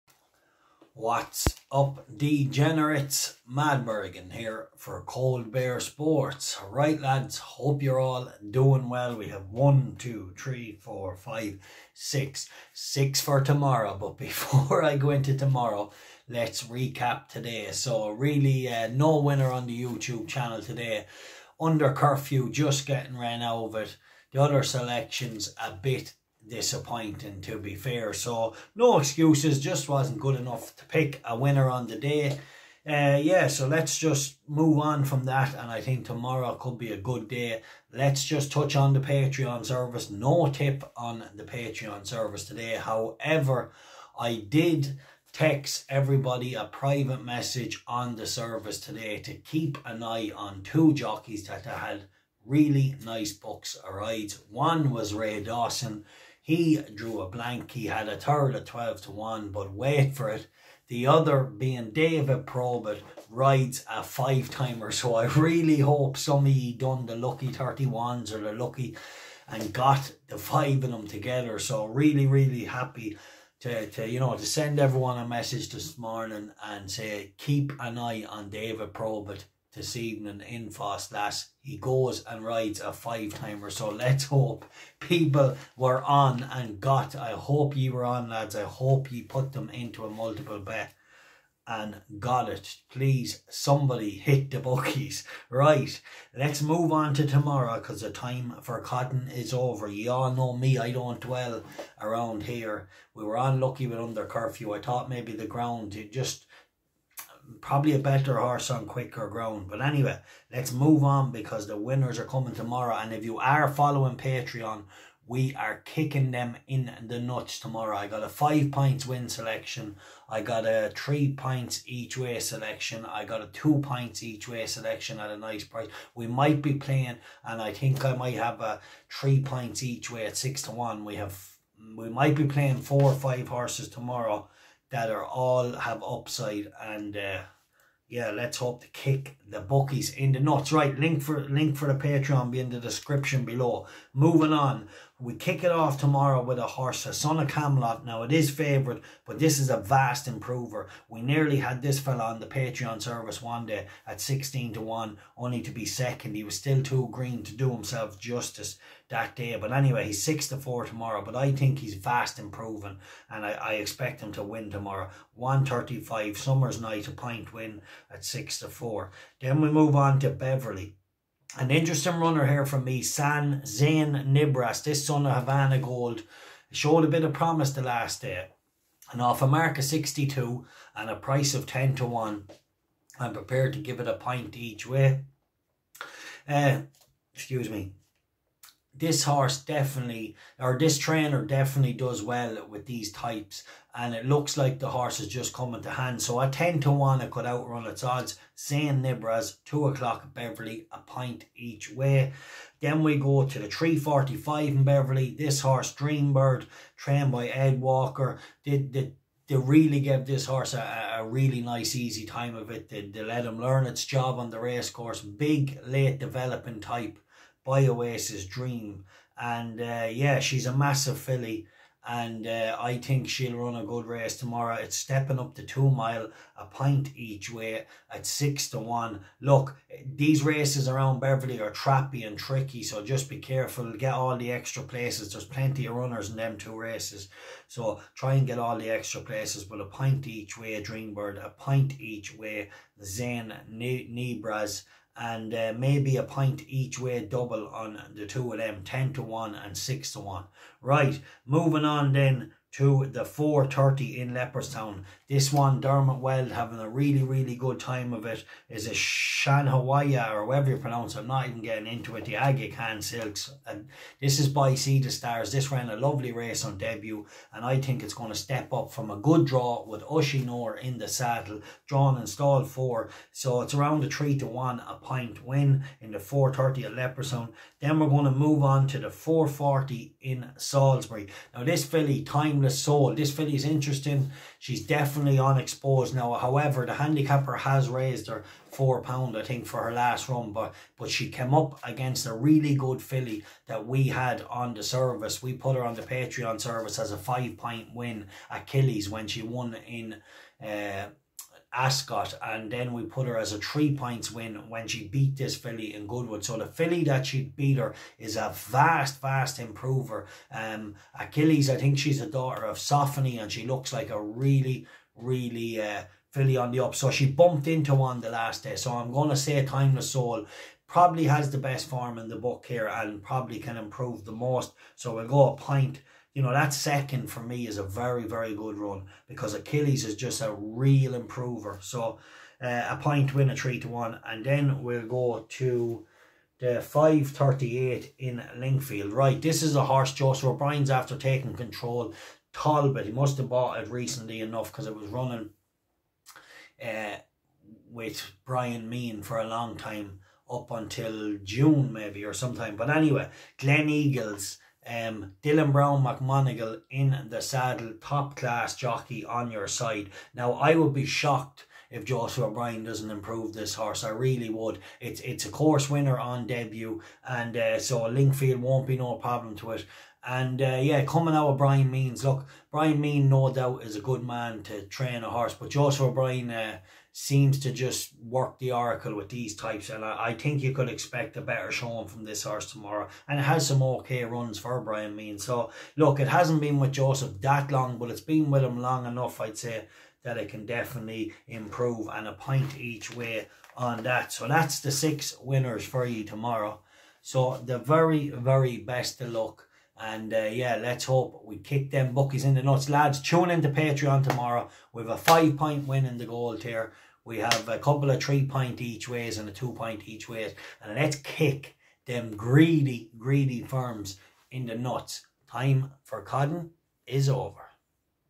uk & irish horse racing tips 🏇 Mad Merrigan UPDATED WINNER 🏆
2.00 Beverley
Zian Nibras 1pt each way 10 to 1

3.45 Beverley
Dreambird 1pt each way 6 to 1
also a 1pt each way double on them two

1.35 FFos las
Summers Knight 1pt win 6 to 4

4.30Leopardstown
Shamiyana 1 pt win 3 to 1

4.40 Salisbury
Timeless soul 1pt win  3 to 1

5.38 Lingfield
Talbot 1pt each way 11 to 2